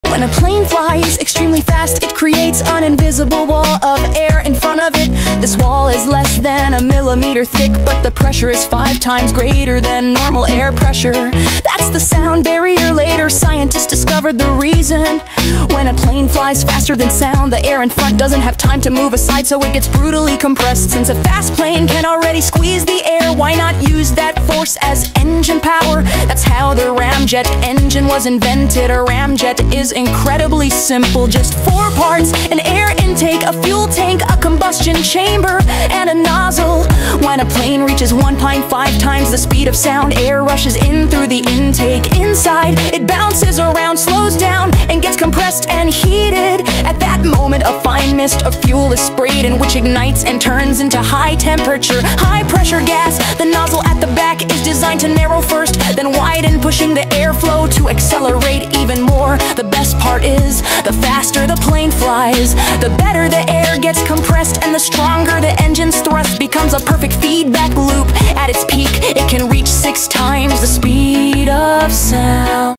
How Jets Break The Sound Barrier 🤔
How do jets break the sound barrier? 💥✈️ Discover the physics of supersonic flight, shock waves, and the engineering power that lets aircraft go faster than sound!